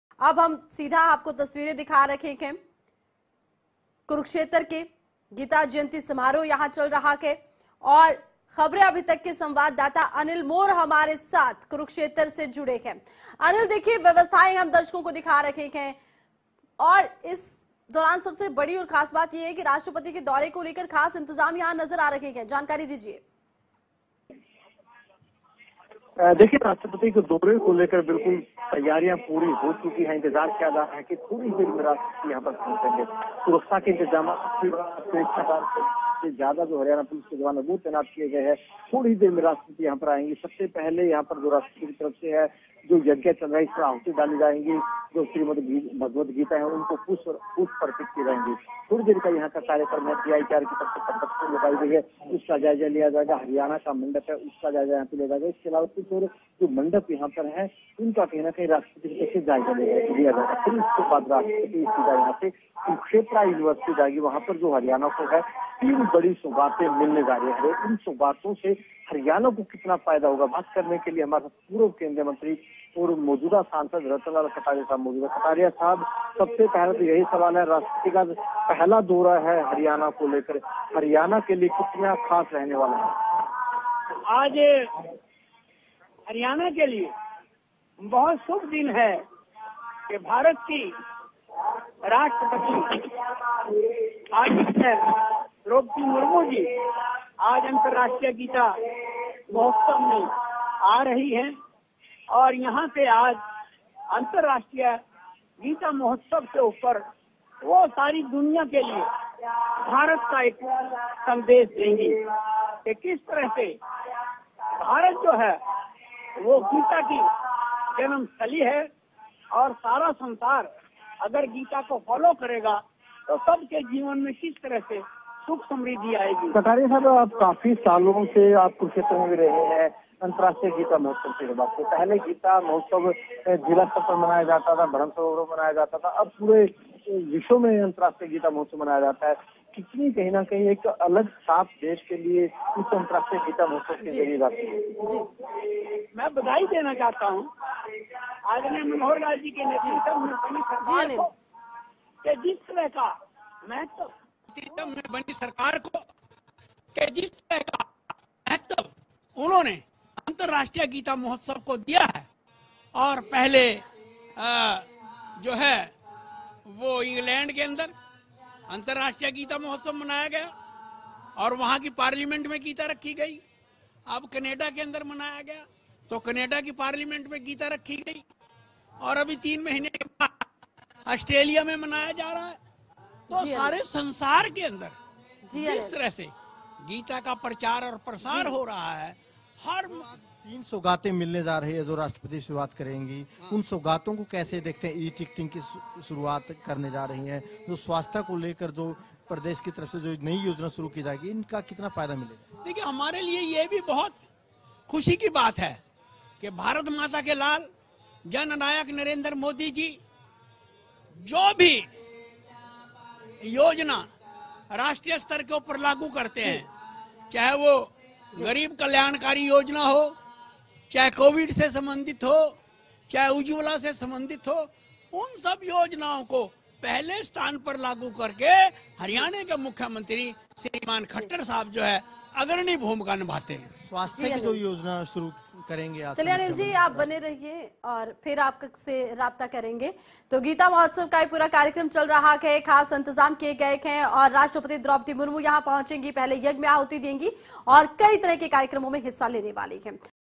Geeta Jayanti Mohatsav 2022: राष्ट्रपति द्रौपदी मुर्मू का कुरुक्षेत्र दौरा कितना खास ? जानिए
राष्ट्रपति द्रौपदी मुर्मू का हरियाणा दौरा, कुरुक्षेत्र गीता महोत्सव कार्यक्रम का करेंगी शिरकत, जानिए महामहिम राष्ट्रपति का दौरा कितना खास ?